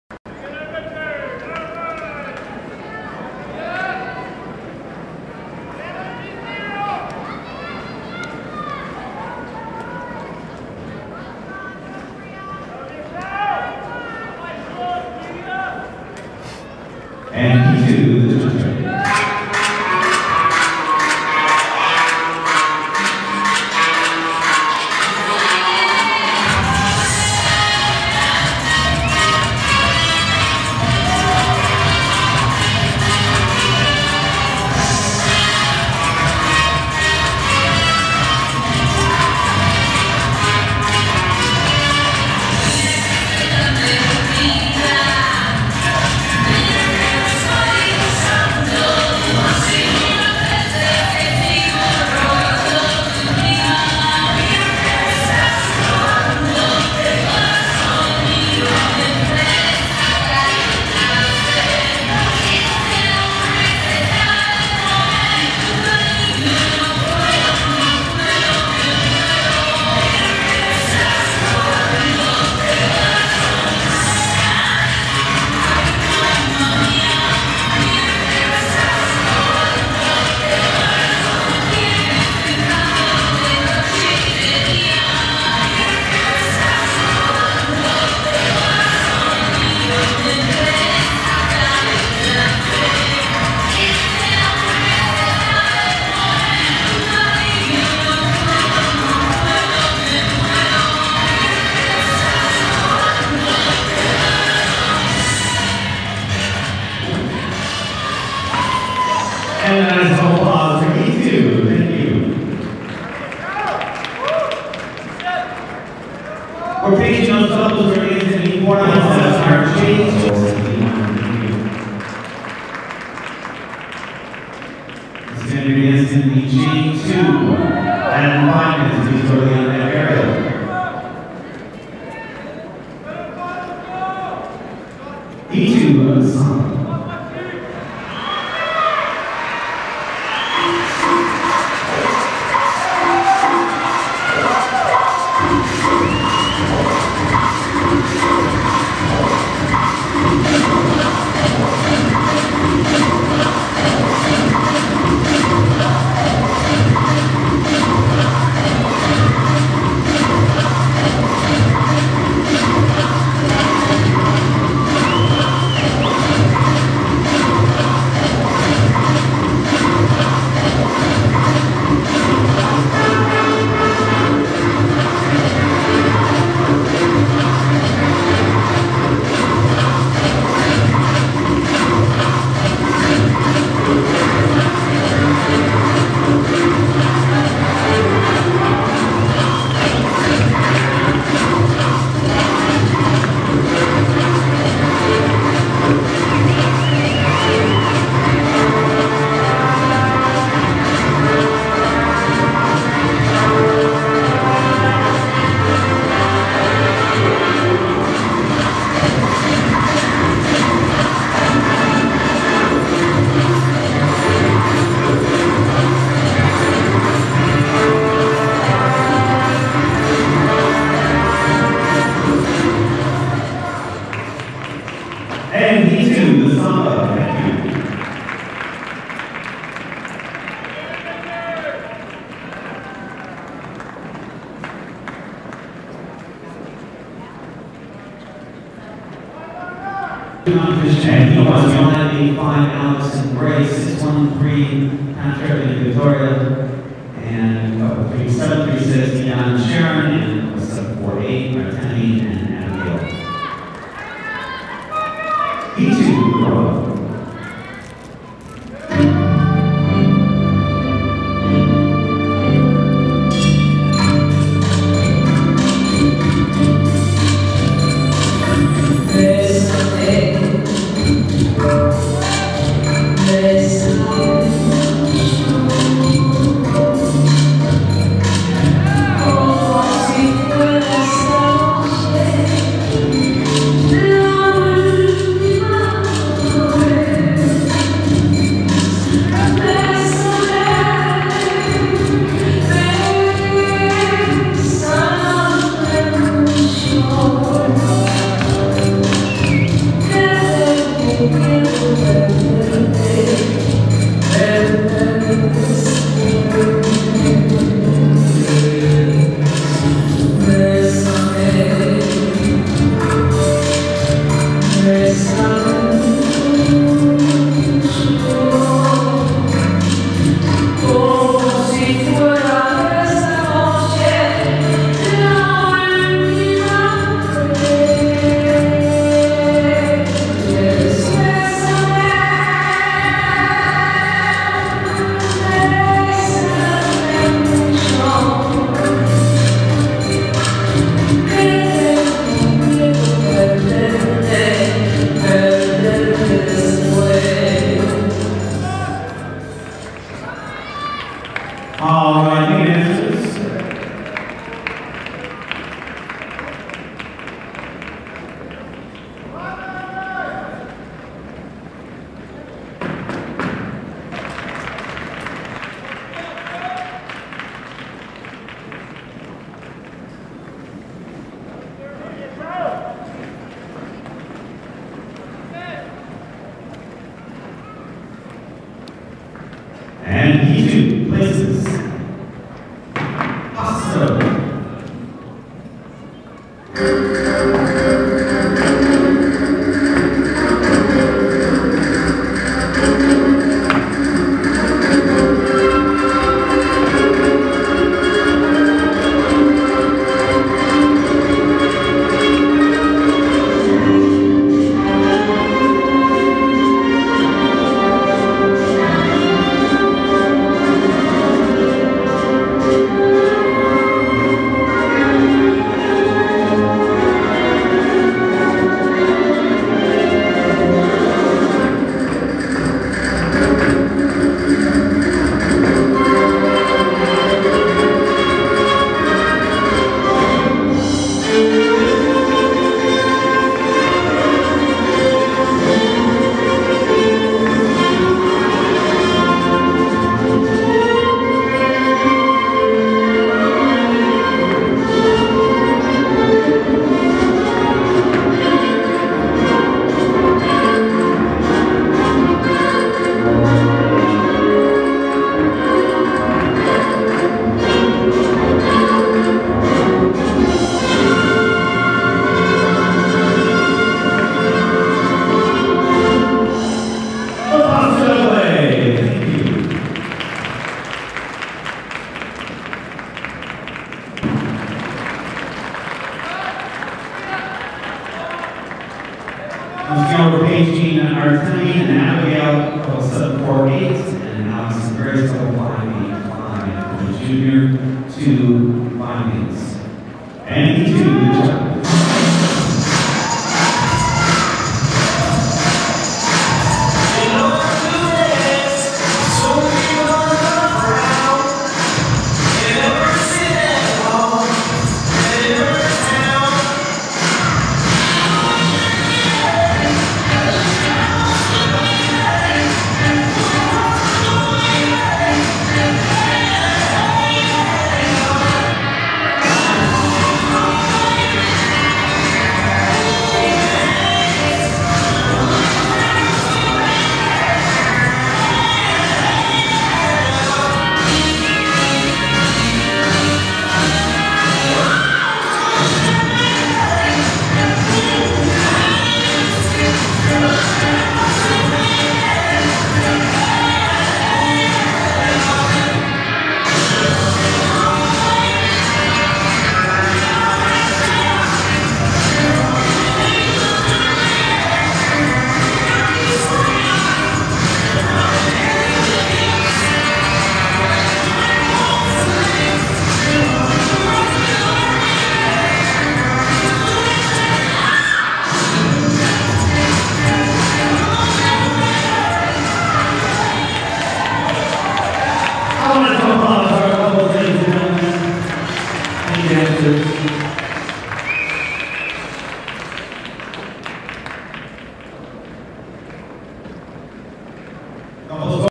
Ohio Star Ball 2012 Youth Championship Latin - Daniel Romaikin and Sabrina Svirskaya
#735; Semi-Final Round of the Youth Latin Championships at Ohio Star Ball in Columbus, Ohio.